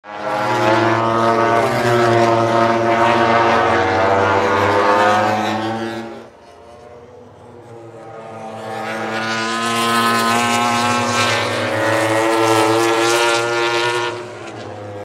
Bradley Smith practice crash | 2020
You can’t park your bike here! 🛑 Bradley Smith walked away from this quick off at the EmiliaRomagnaGP in 2020 👍